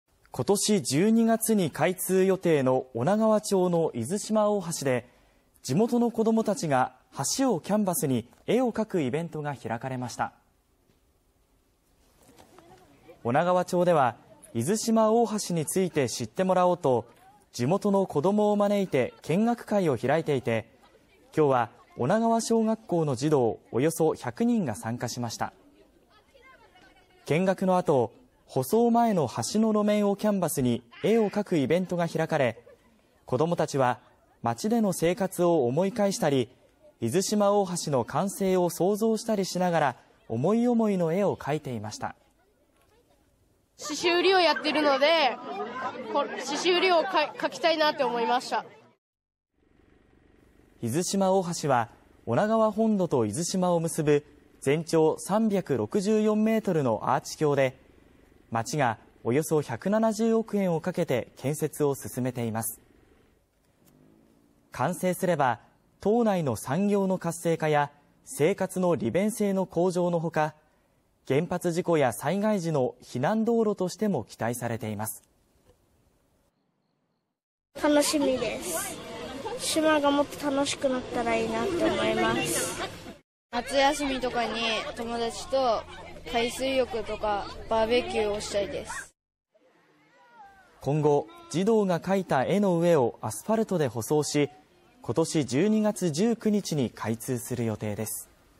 児童が橋をキャンバスに絵を描く １２月開通予定の宮城・女川町出島大橋
１２月に開通予定の宮城県女川町の出島大橋で、地元の子どもたちが橋をキャンバスに絵を描くイベントが開催されました。